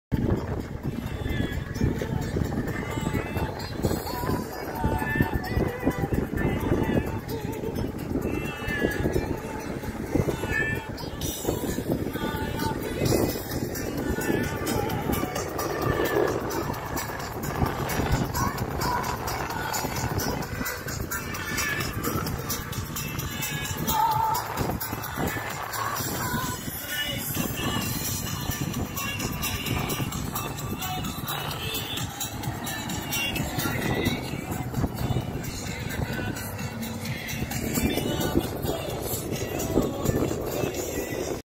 Snorkeling trip to the dolphin house from Soma bay by speedboat rental
Dolphins Jumping Out of the Water in Hurghada, our tour by speedboatrental with my family more @